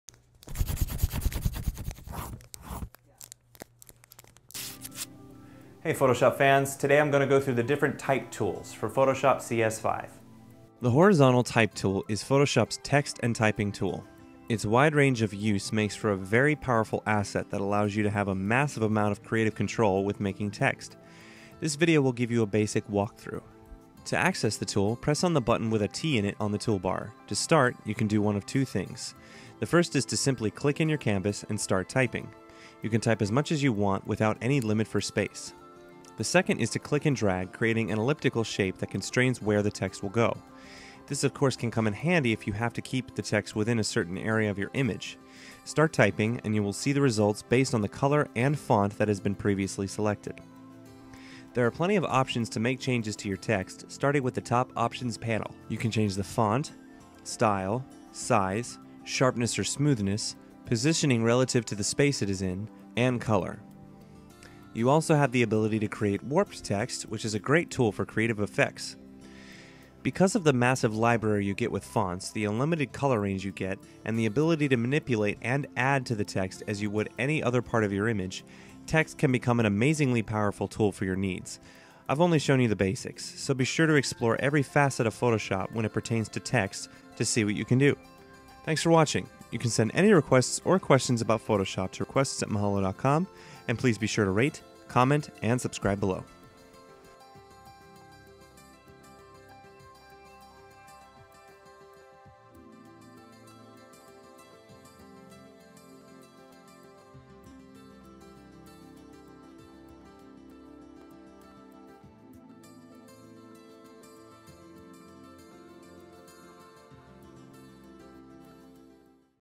Learn Adobe Photoshop - Horizontal Type Tool
Mahalo expert Justin Z. gives a tutorial on the Photoshop horizontal type tool.

About the Horizontal Type Tool

The Horizontal Type Tool is Photoshop's text and typing tool. It's wide range of use gives you a massive amount of creative control when incorporating text into your images.

Using the Horizontal Type Tool

1. To access this feature, click on the T icon in the
Tool bar (leftmost side of your screen).
2. Click anywhere in your canvas and you can start typing. You can type as much as you want without any limit for space. You may also click and drag across the canvas, which will create a shape in which your text will be written. This can come in handy if you need to restrict a designated area for your text within the image.
3. To alter the font style, size, color, sharpness & smoothness and, lastly, the positioning of the text relative to the space it is in, choose any of the features within the top Options Panel. You can also choose "Warped Text," which is a great tool for creative text effects.
4. Click the warped T icon in the Options Panel and it will prompt the Warped Text window. Here you can choose a fun variety of warped effects for your text. These are just the basics of the Horizontal Type Tool. With a little practice, you can implement some powerful written additions to your graphic designs.